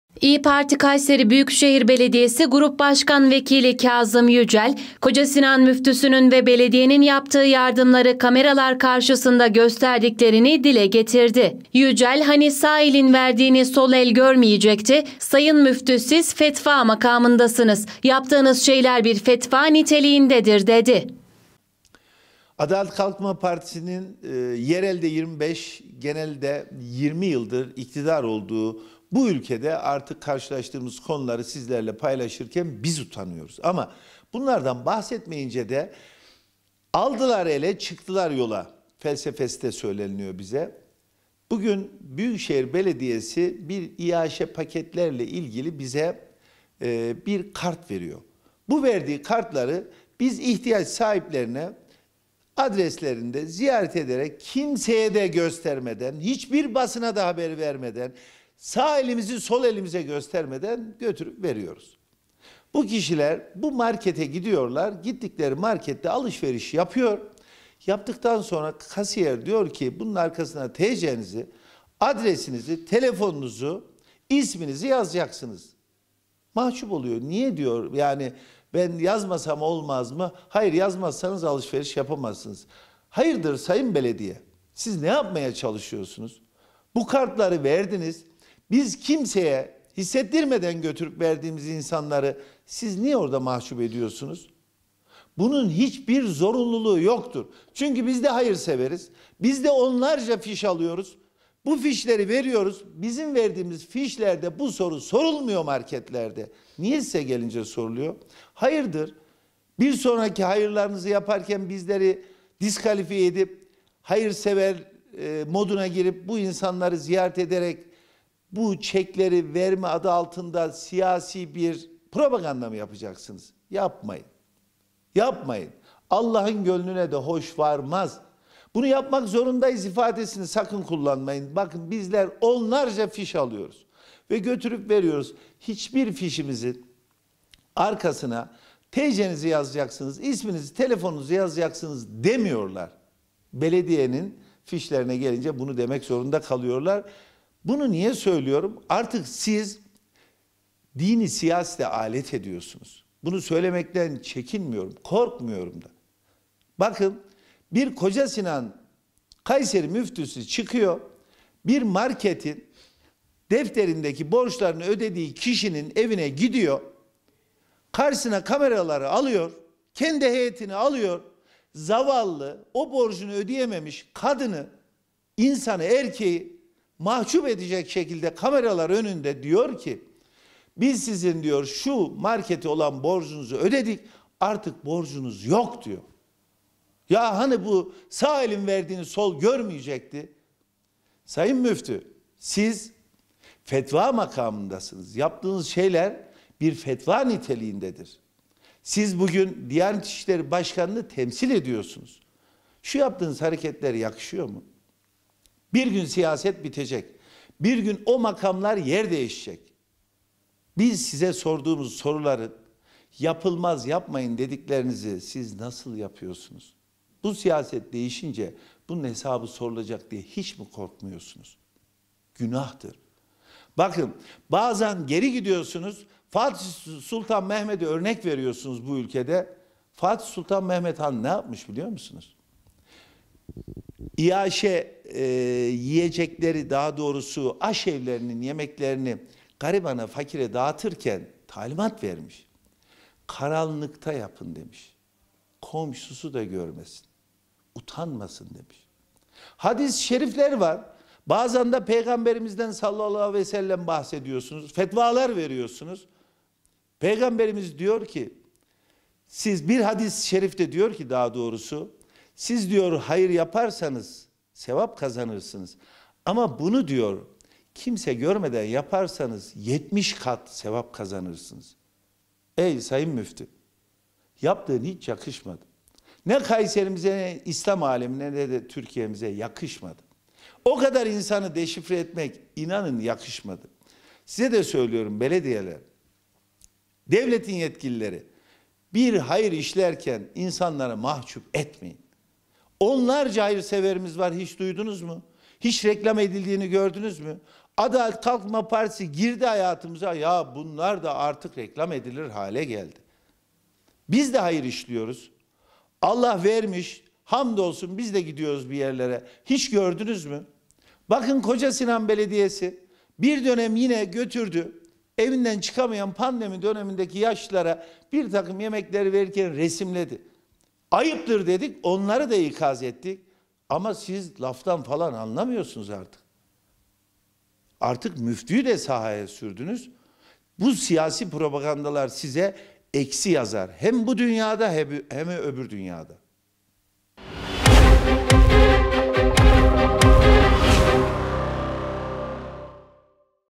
İYİ Partili Yücel: “Hani sağ elin verdiğini sol el görmeyecekti”
KAYSERİ (1HA) – İYİ Parti Kayseri Büyükşehir Belediyesi Grup Başkanvekili Kazım Yücel, Kocasinan Müftüsünün ve Belediyenin yaptığı yardımları kameralar karşısında gösterdiklerini dile getirdi. Yücel, “Hani sağ elin verdiğini sol el görmeyecekti. Sayın müftü siz fetva makamındasınız yaptığınız şeyler bir fetva niteliğindedir” dedi.

Kayseri Belediyesinin ve Kocasinan Müftüsü Halil Uzun’un geçtiğimiz gün bir vatandaşın market borcunu ödemesi hakkında konuşan Yücel, “Bu kişiler markete gidiyor alışverişlerini yapıyor, yaptıktan sonra da kasiyer diyor ki arkasına TC’nizi, numaranızı, adresinizi yazacaksınız. Yamaksak ne olur deyince de, hayır yazmazsanız alışveriş yapamazsınız diyorlar. Sayın belediye siz ne yapmaya çalışıyorsunuz. Biz kimseye haber vermeden, hissettirmeden verdiğimiz ihtiyaç sahibi insanları niye mahcup ediyorsunuz” dedi. Kendilerinin verdiği fişlerde bu soruların sorulmadığını söyleyen Yücel, iktidarı dini siyasete alet etmekle nitelendirdi.

Yücel, “Bakın bir Kocasinan Kayseri müftüsü çıkıyor bir marketin defterindeki borçlarını ödediği kişinin evine gidiyor, karşısına alıyor kameraları, kendi heyetini alıyor zavallı borcunu ödeyememiş insanı karşısına alıyor kameralar önünde diyor ki. Biz sizin markete olan borcunuzu ödedik artık sizin borcunuz yok diyor. Hani sağ elin verdiğini sol el görmeyecekti. Sayın müftü siz fetva makamındasınız yaptığınız şeyler bir fetva niteliğindedir. Siz bugün Diyanet işlerini temsil ediyorsunuz, yaptığınız yakışıyor mu” dedi. Fatih Sultan Mehmet’in o zamanlar yaptığı yardımlarda karanlıkta yardım edilmesini istediğini vurgulayan Yücel, Peygamberimizin hadislerinden de örnekler verdi.
Yaptığınız yakışmadı ifadelerini kullanan Yücel,” Onlarca hayırsever var hiçbirini duydunuz mu, reklam edildiğini gördünüz mü? Artık müftüyü de sahaya sürdünüz, bu siyasal propagandalar size eksi yazar hem bu dünyada hem diğer dünyada” dedi.